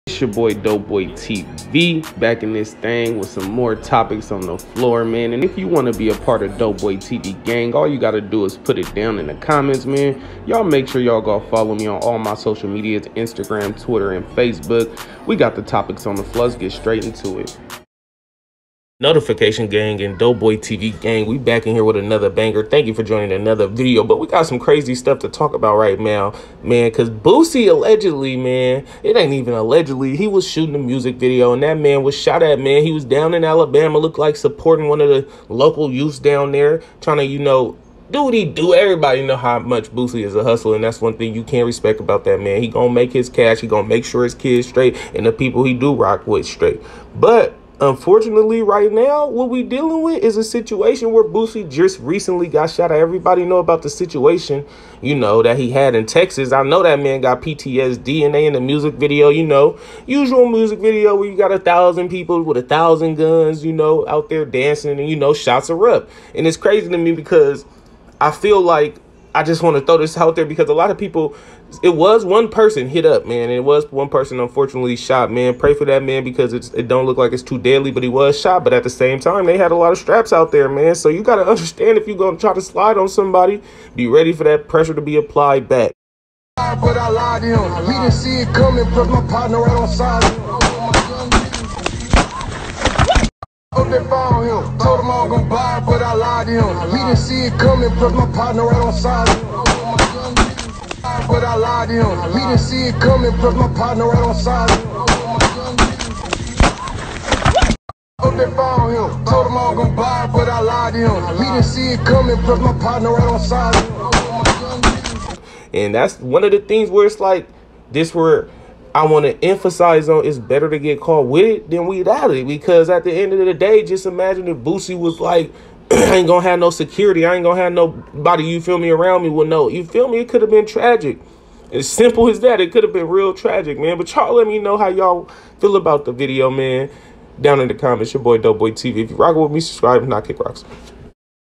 Boosie Music Videos Shot 🆙 In Alabama “Everyone RAN🏃🏾4 Cover” 1 Person HIT Condition UnKnown 🙏🏾
THANKS🙏🏾 FOR ALL THE LOVE🥰 AND
SUPPORT💪🏾💯🙌🏾🦍🦍🦍

WE STEPPIN OUT HERE